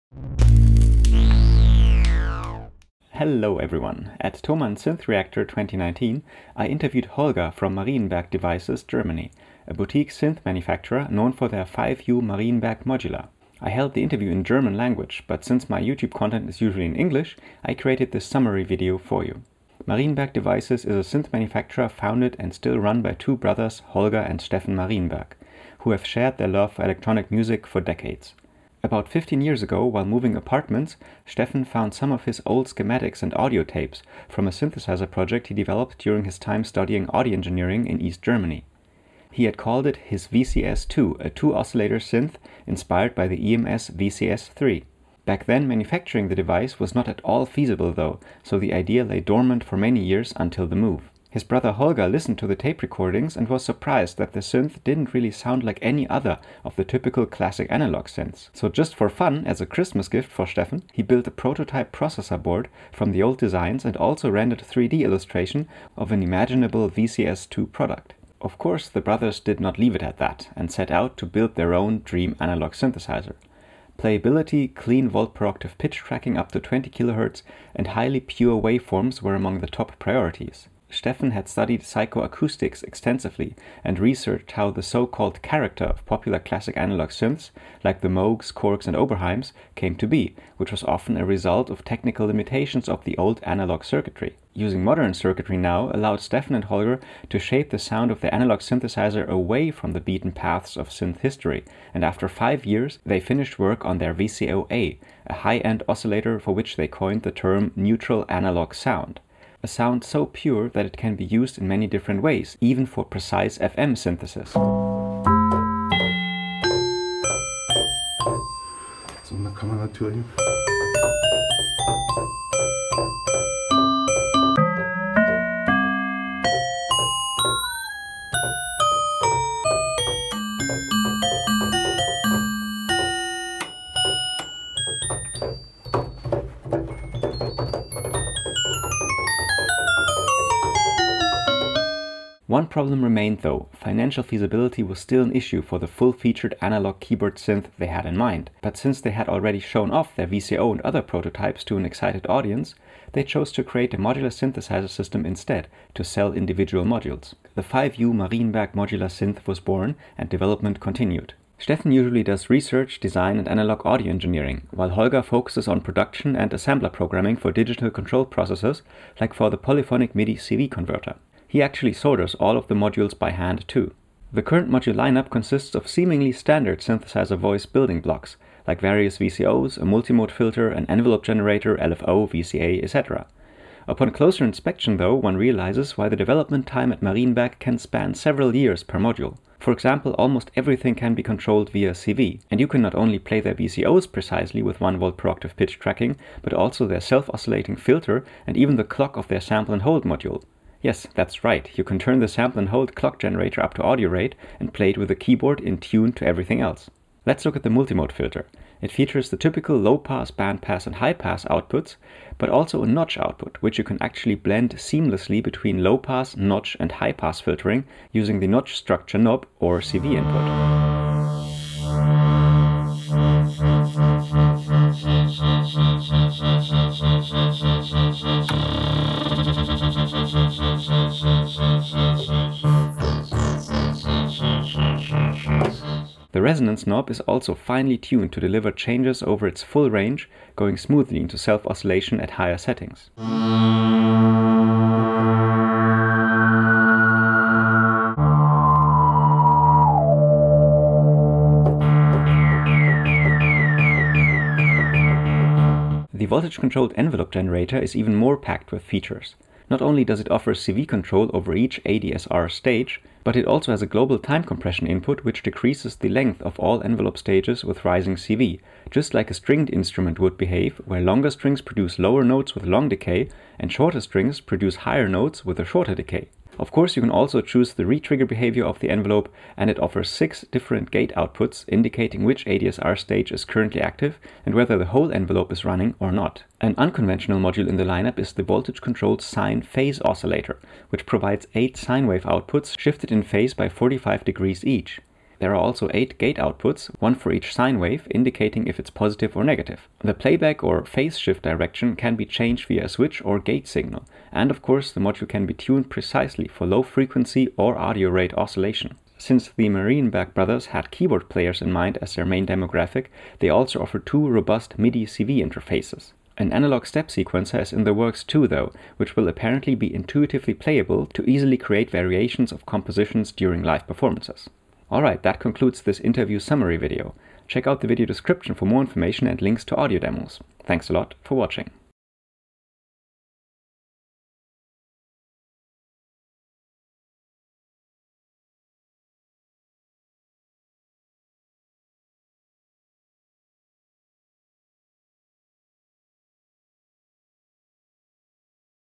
Marienberg Modular Synth Interview Summary (ENGLISH)
At Thomann's Synth Reactor 2019 I had the unique opportunity to meet and talk to Steffen and Holger Marienberg extensively. I filmed an interview with Holger about the origins of the the company Marienberg Devices Germany, in which he also demonstrated some of their 5U modular synth modules. This interview was recorded in German language though, so I made this summary video here for my English viewers =)

If you've read this far, post a comment with the words "frequency modulator" in it :D